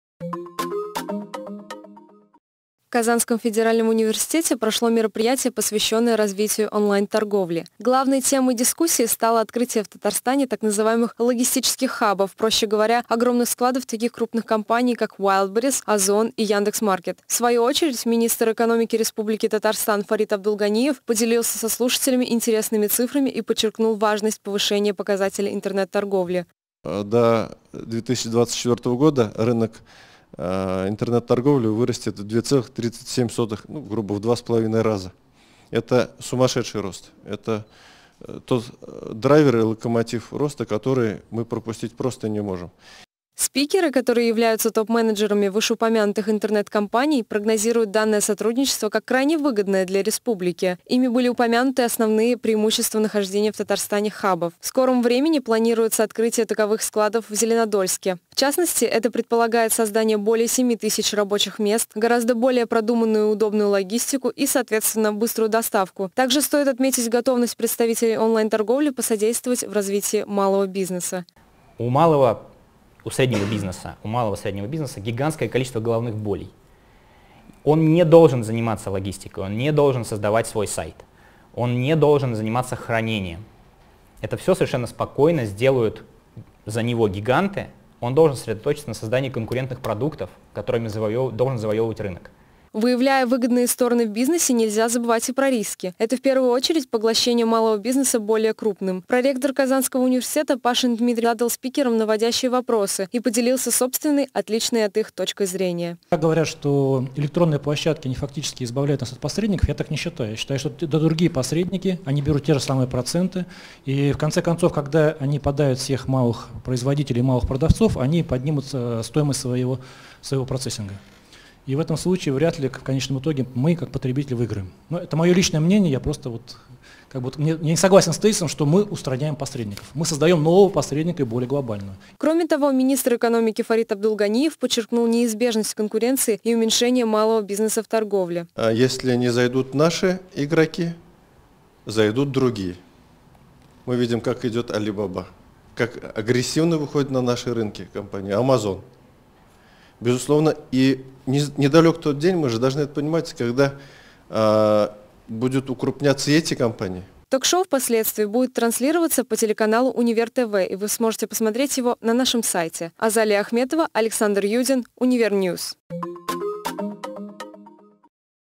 ПЕРСПЕКТИВЫ РОЗНИЧНОЙ ОНЛАЙН-ТОРГОВЛИ
В Казанском федеральном университете прошел дискуссионный  клуб  КФУ на тему: "Розничная   онлайн - торговля. Перспективы глобальные, национальные  и региональные